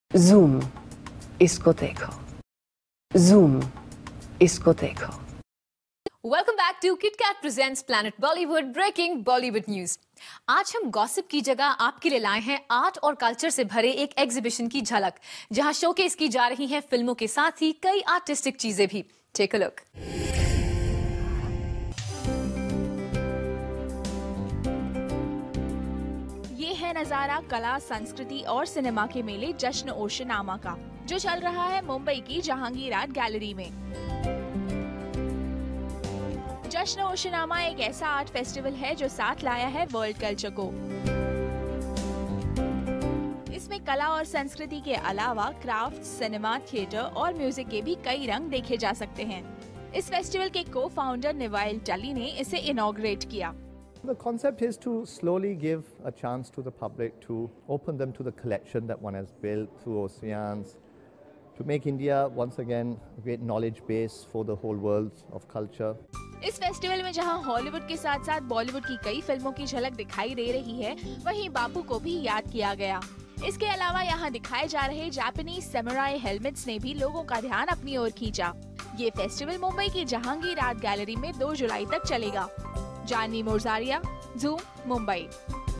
Movies on canvas
Zoom had recently visited a cultural fest titled Jashn Osianama at the Jehangir Art Gallery in Mumbai which showcased a wide variety of collections from various places. The list of collection includes pieces from Tibetan Thangkas to Japanese Samurai Art, from film memorabilia to the best of modern and contemporary art form. Besides art and cultures, new faces of craft, cinema, theatre and music are also revealed at this fest.
The co-founder of the fest Neville Tuli inaugurated the fest and said that the whole concept of putting up a event was to give the public a chance to open up to the collection that one has built and to make India a knowledge base of cultures once again. These collectibles are not for sale and is up for preview only till 2nd of July. All this and moretake a look!